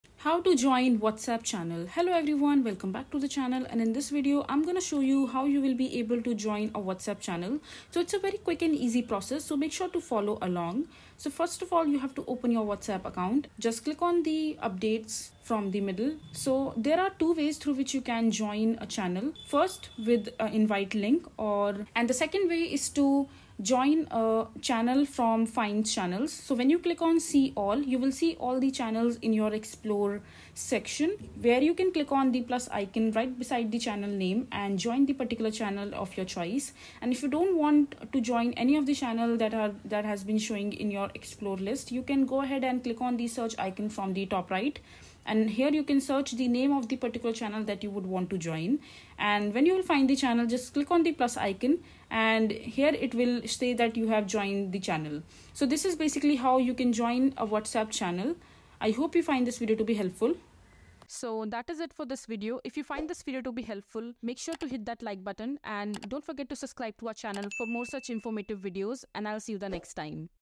How to join WhatsApp channel (Full Guide)
In this video, we'll show you how to join a WhatsApp channel. This full guide will show you how to create a new WhatsApp channel, add members, and share files.

WhatsApp is a popular messaging app that has gained a lot of popularity in the past few years. If you're looking to join a WhatsApp channel, this guide will show you everything you need to know! We'll guide you through the process of creating a new channel, adding members, and sharing files.